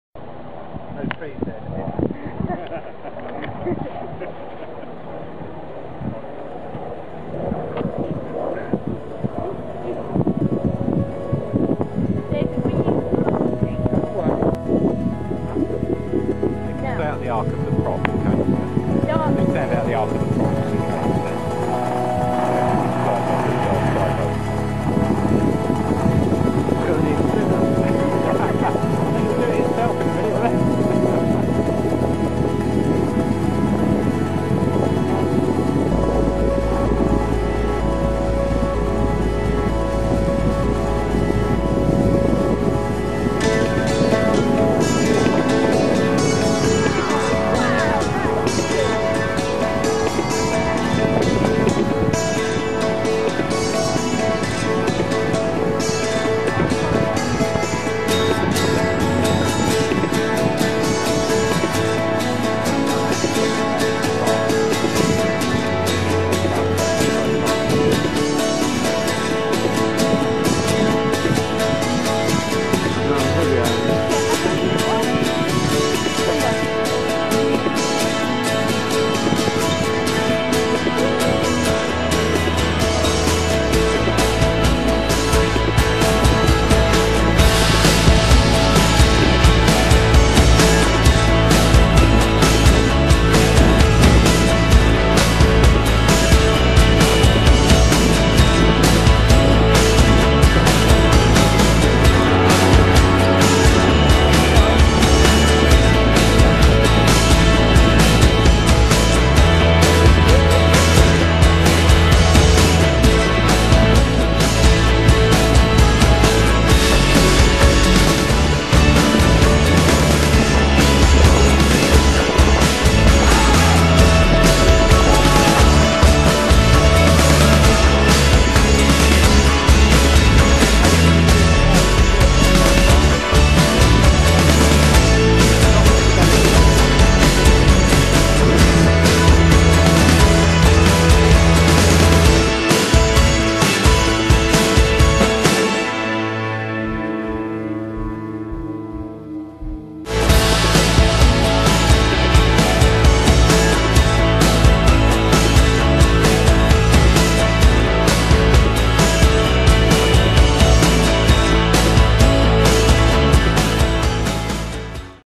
Jerry's Gladiator
This Is Not My Model, I Recorded It and Uploaded It for Jerry
This is a video of a model built by a fellow club member
The model is a scratch built from plan Gloucester Gladiator
Built and Flown by Jerry Frost